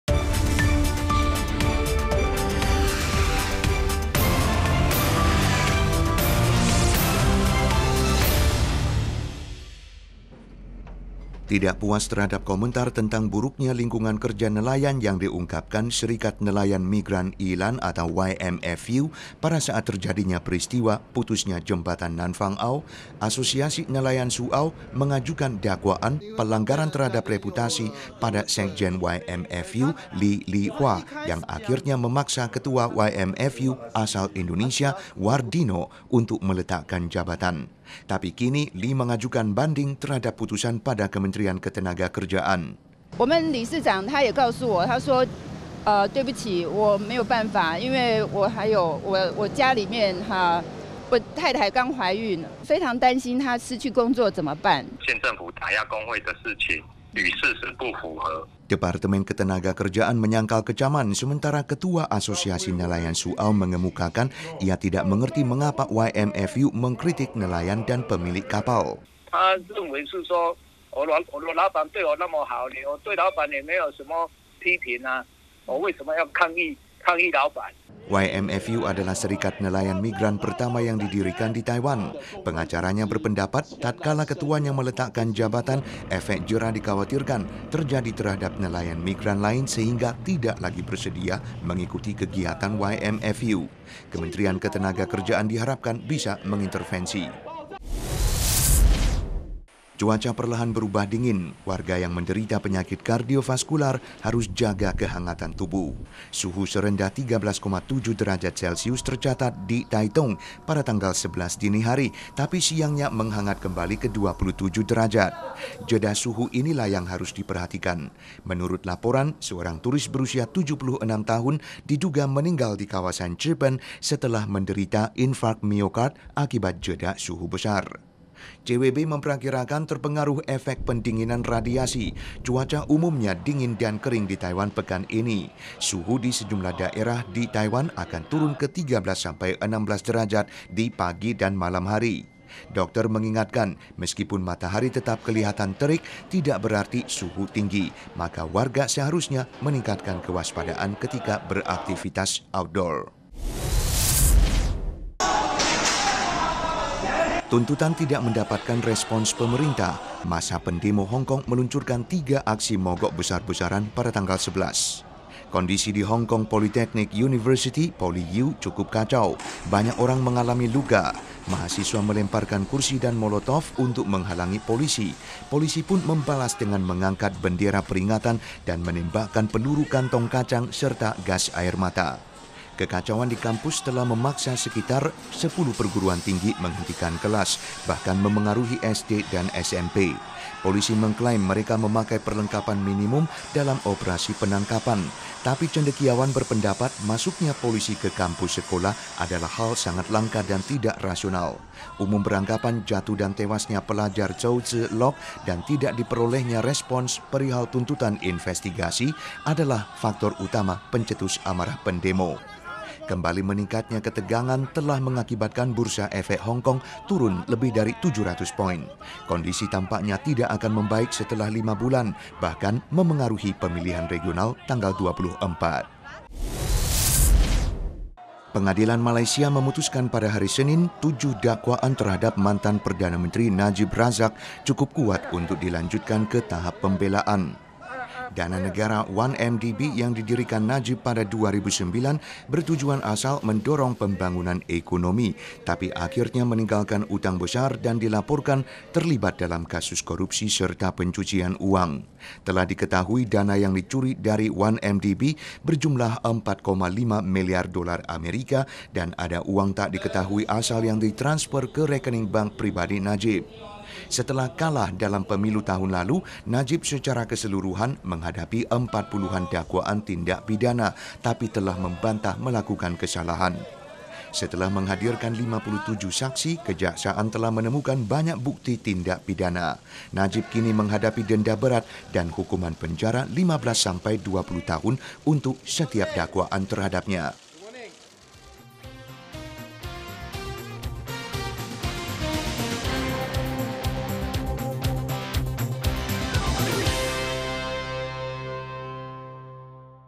20191112 Warta Berita PTS Taiwan公視印尼語新聞
Serikat Nelayan ajukan banding terhadap dakwaan Asosiasi Nelayan

Penderita kardiovaskular diingatkan mengawasi jeda suhu besar

Pendemo HK luncurkan tiga aksi mogok besar-besaran

Tujuh dakwaan terhadap Najib Razak memasuki pembelaan